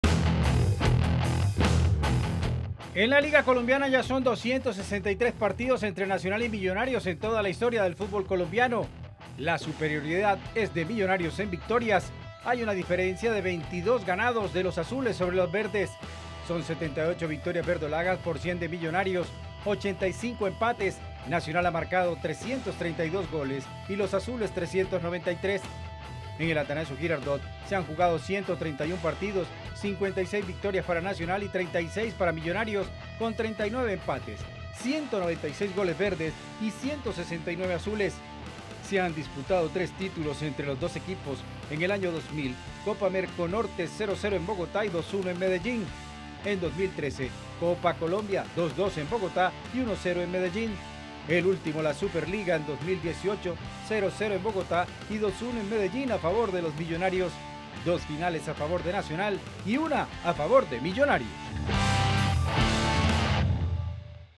Histórico de enfrentamientos - Telemedellín
En la Liga colombiana ya son 263 partidos entre Nacional y Millonarios en toda la historia del fútbol colombiano, la superioridad es de Millonarios en victoria, hay una diferencia de 22 victorias de los azules sobre los verdes., son 78 victorias verdolagas por 100 de Millonarios, 85 empates, Nacional ha marcado 332 goles y los azules 393.